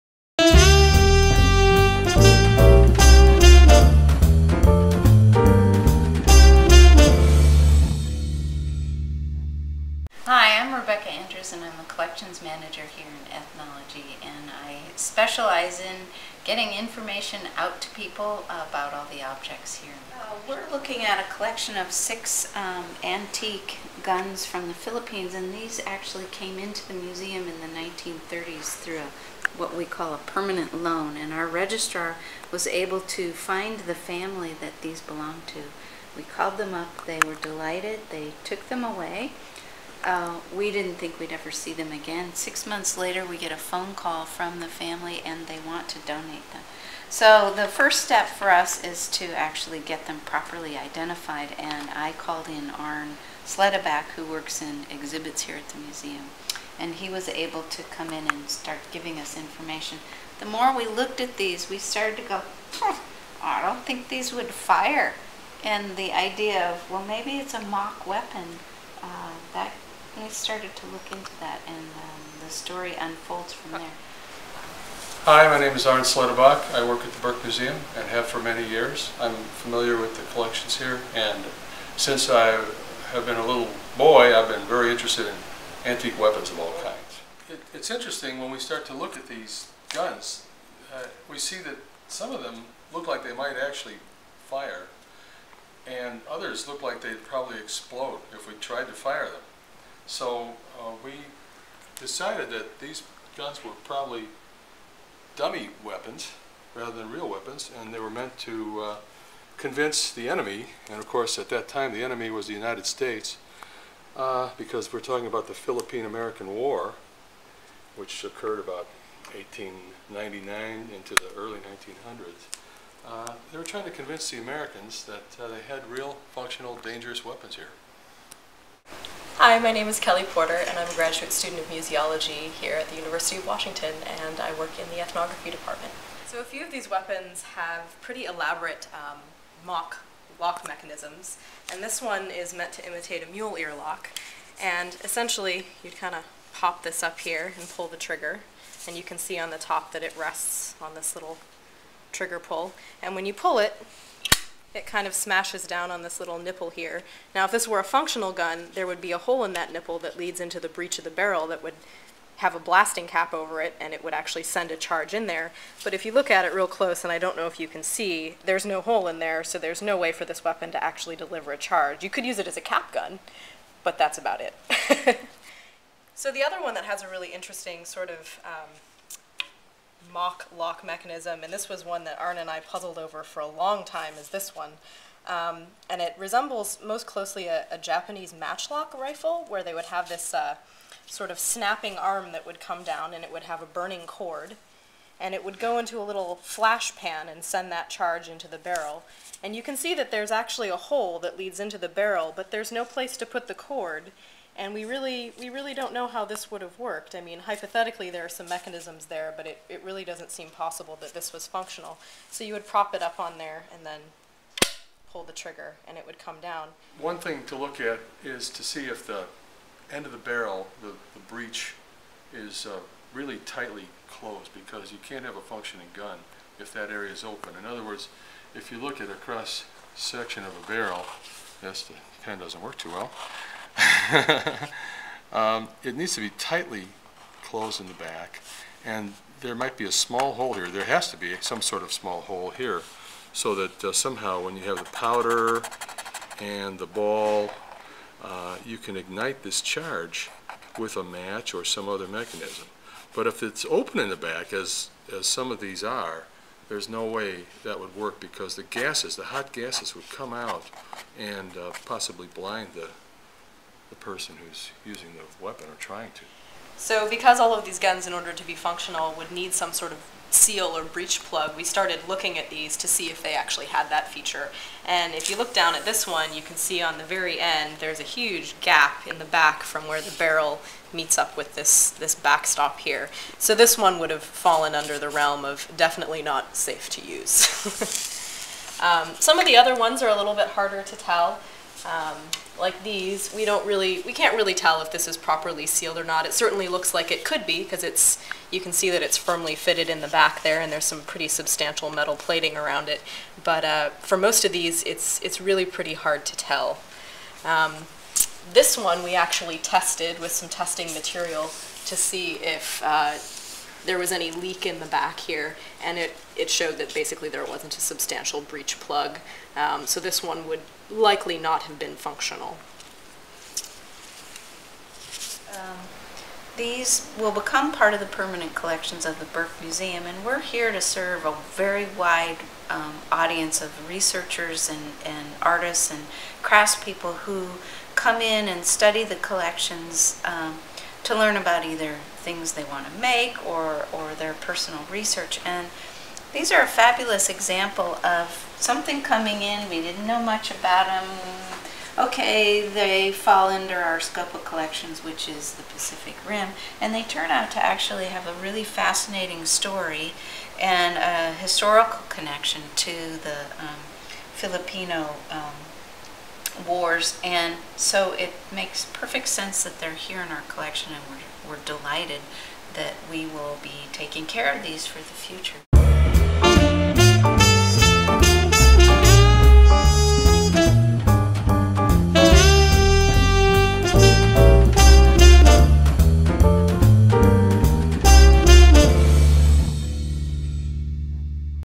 The Burke Museum Ethnology Detectives
Staff members at the Burke Museum of Natural History and Culture discuss their research on a set of antique dummy rifles from the Philippine-American War.